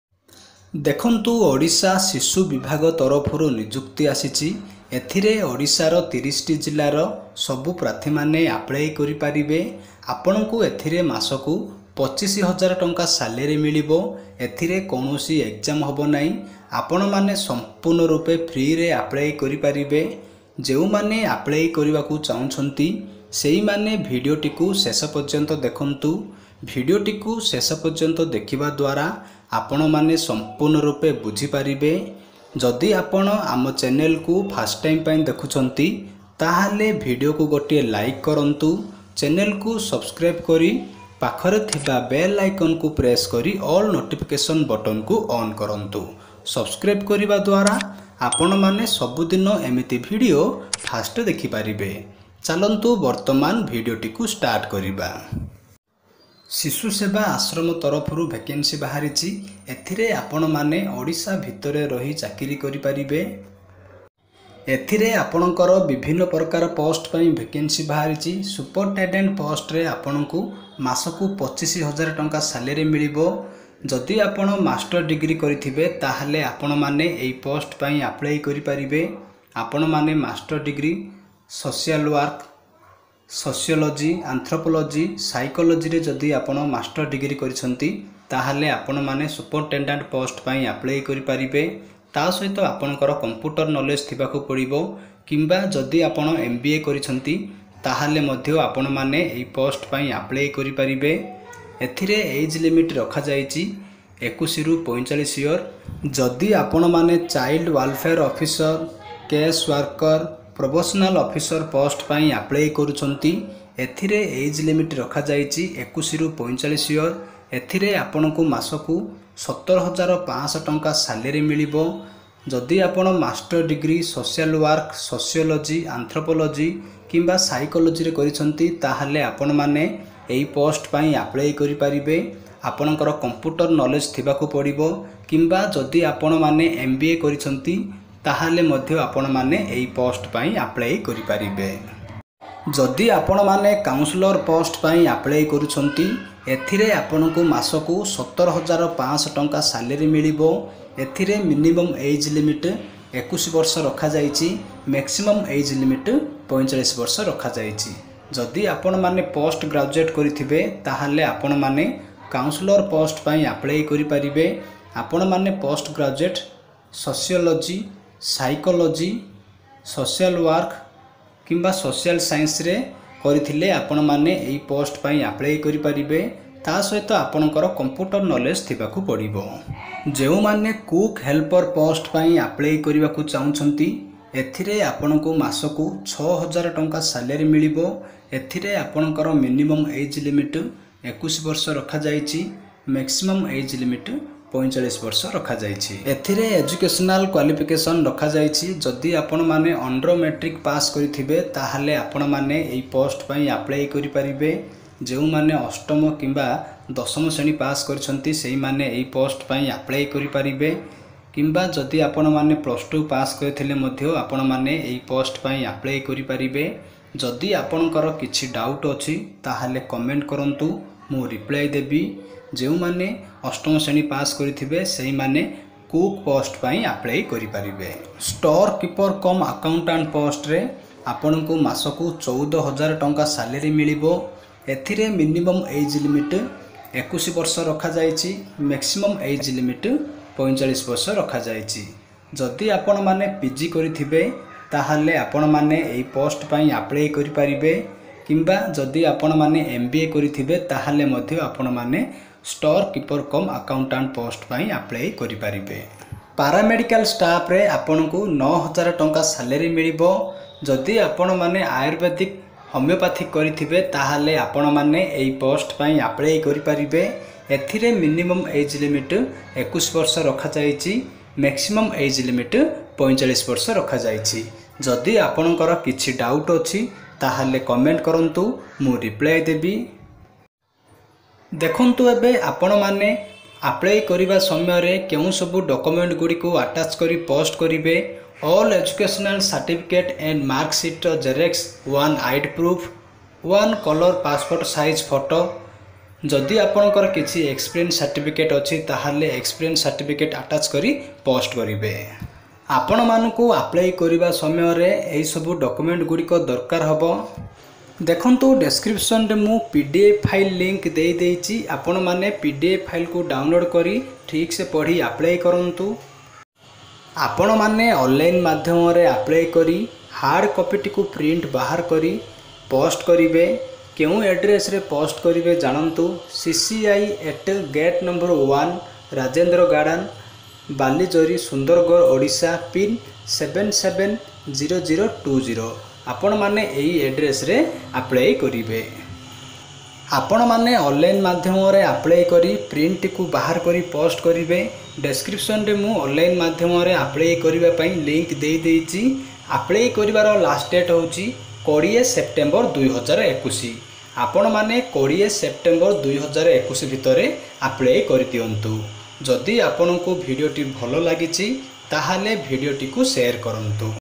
ଓଡିଶା ଶିଶୁ ବିଭାଗରେ ବଡ଼ ନିଯୁକ୍ତି. ଦରମା:- ୨୫ ହଜାର || Odisha Village And Panchayat Level Job 2021
Odisha sishu seva ashram recruitment 2021.

Odisha village level job 2021.

Odisha Gram Panchayat level job 2021.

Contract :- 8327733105

MAHESWAR SETHI